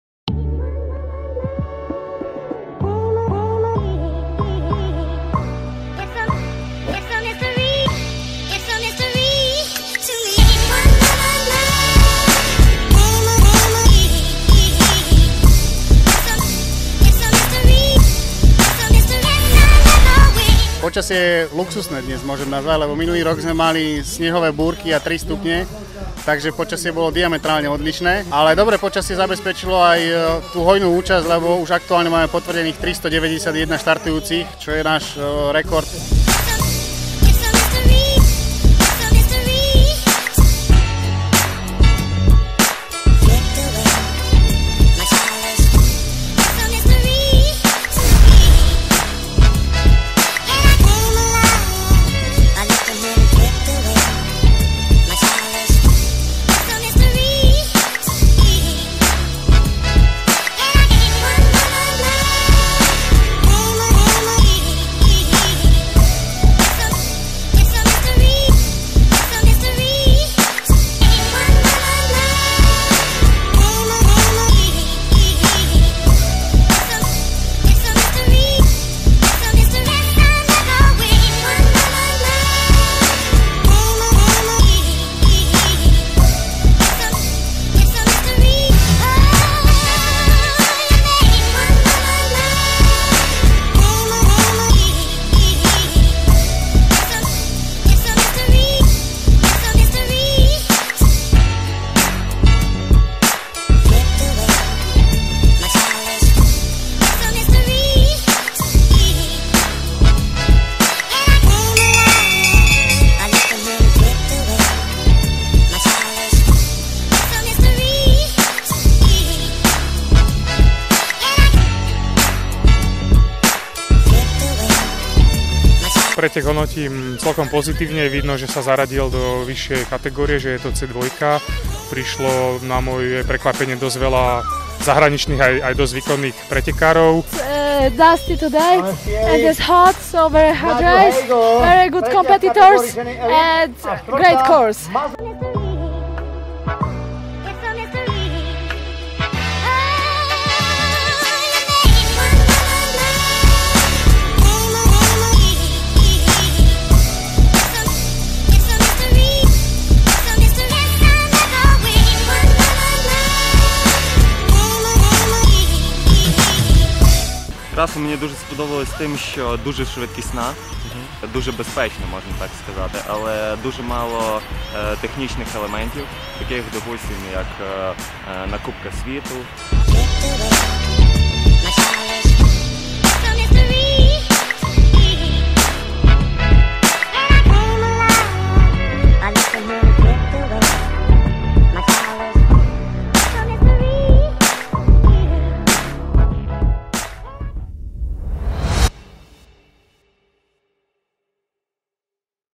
TURIECKAP 2018 official aftermovie - MTB XCO UCI C2 1. kolo SP
Pozrite si to najlepšie z pretekov MTB XCO UCI C2 Turieckap 2018 1.kolo SP.

Kamera: Vladyslav Naumeniuk, Michal Čabai
Strih: Michal Čabai
Za výpomoc ďakujem: Veronika Ronie Vozárová, Veronika Koniarová, Nazar Struk, Tomáš Hauser